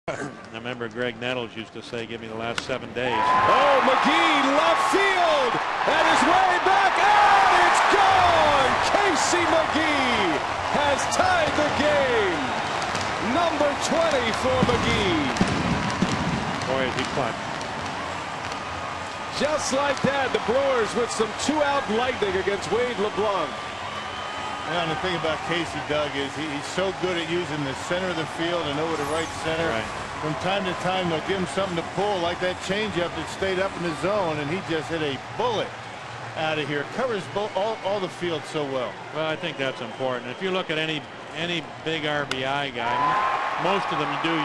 McGehee's three-run jack
SD@MIL: McGehee ties the game with three-run blast